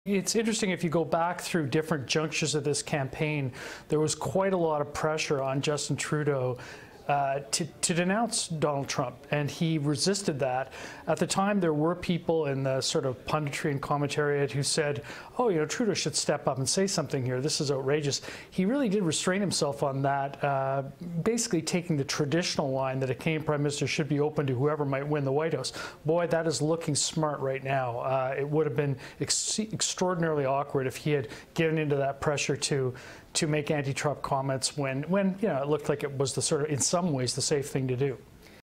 Election, Eh: Trudeau was smart not to bash Trump, Geddes says
Maclean's Ottawa bureau chief looks at the way Justin Trudeau spoke about Trump during the election.